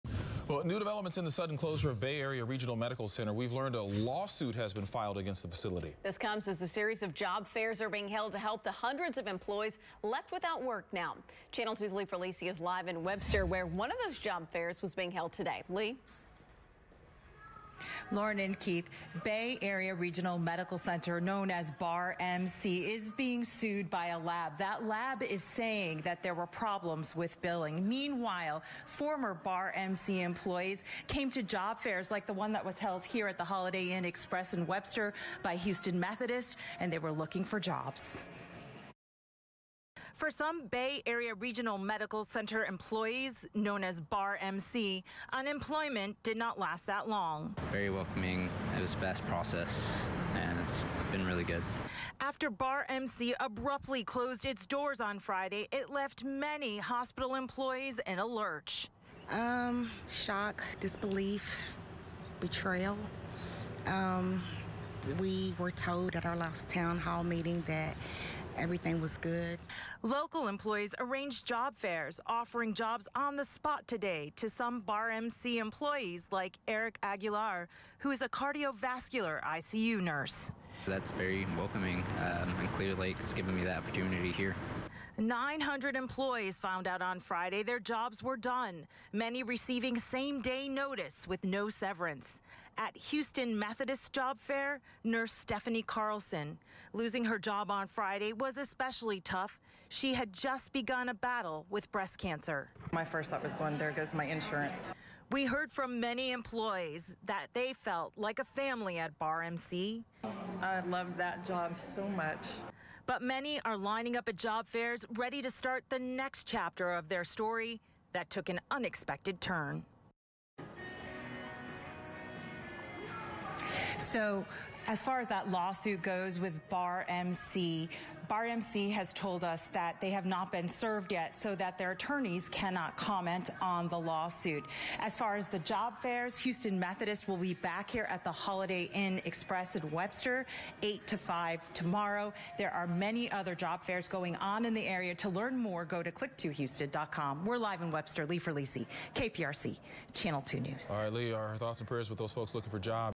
Bay Area Regional Medical Center sued
Bay Area Regional Medical Center, in Webster, announced it will close its doors and file for bankruptcy, according to a news release from Bay Area Regional Medical Center LLC.